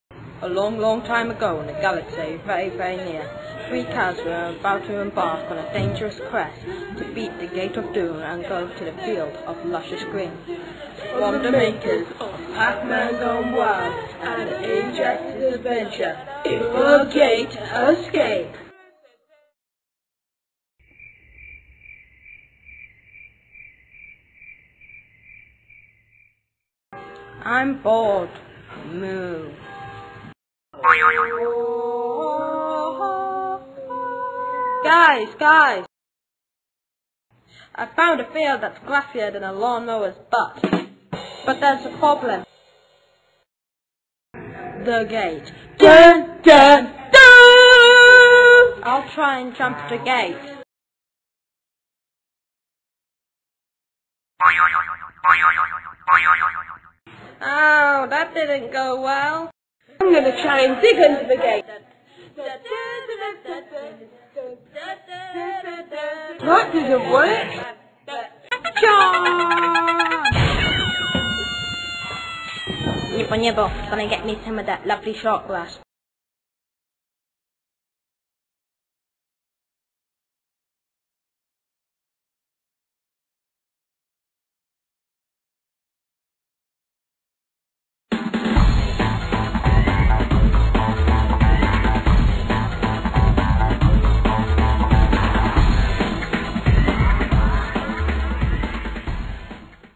The Gate Escape
This animation was made at the Flash Animation Club at the Tyneside Cinema in 2008 - Tutored by Robin Webb